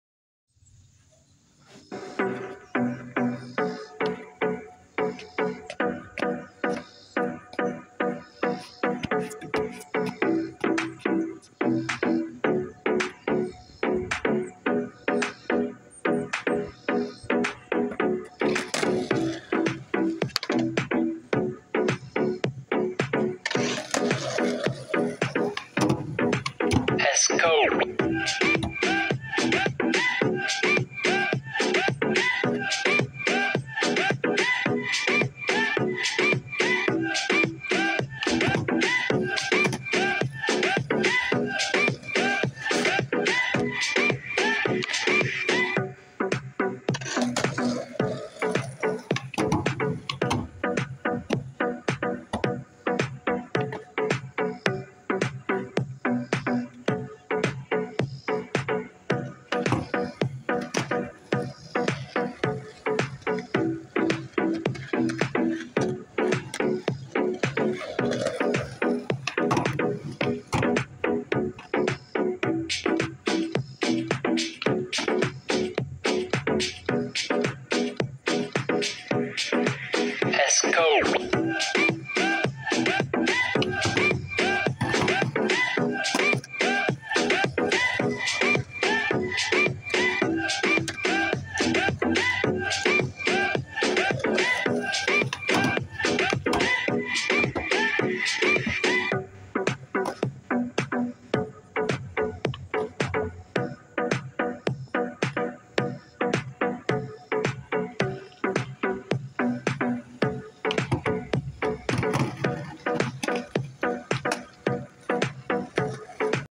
Skatin At Franklorino Skatepark Part 1 😊
I'm Having Fun Skateboarding At Franklorino Skatepark In Morristown Tennessee 😊